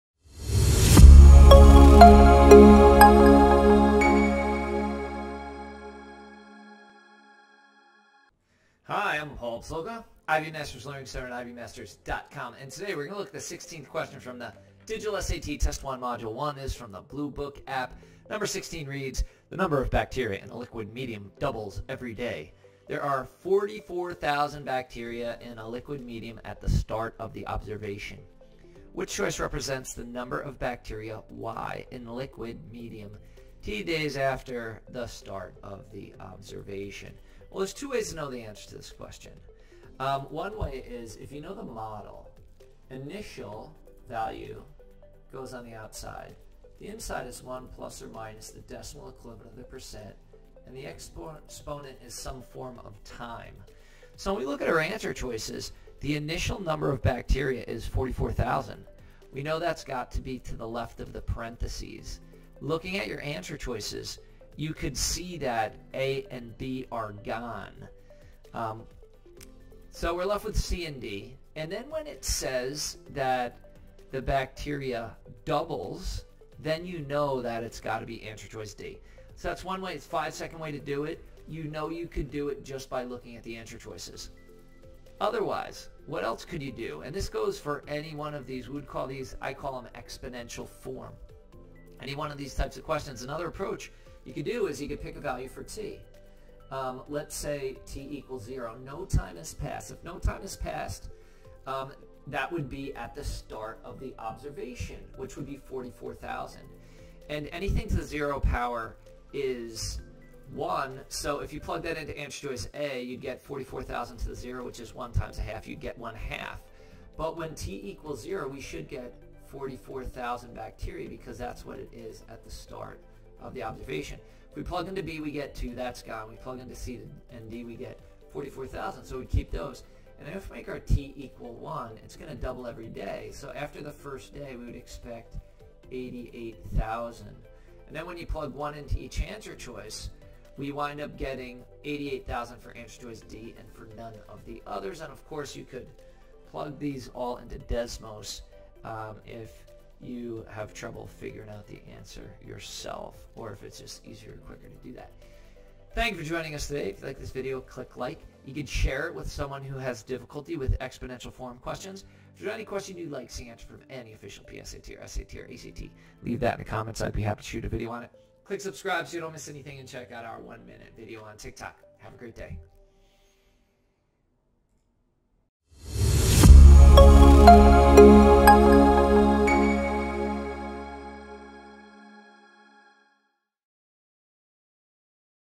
Digital SAT math practice test 1, module 1, number 16 || Ivy Masters
How to solve this exponential form problem in 5 seconds

Bluebook app, Blue Book app

The number of bacteria in a liquid medium doubles every day. There are 44,000 bacteria
in the liquid medium at the start of an observation. Which represents the number of
bacteria, y, in the liquid medium t days after the start of the observation?

(A) y = ½(44,000)^t
(B) y = 2(44, 000)^t
(D) y = 44,000(2)^t

new SAT

Prepare for the SAT Math section with this digital practice test question from module 1, number 16. Join Ivy Masters for expert tips and explanations to help you ace the exam.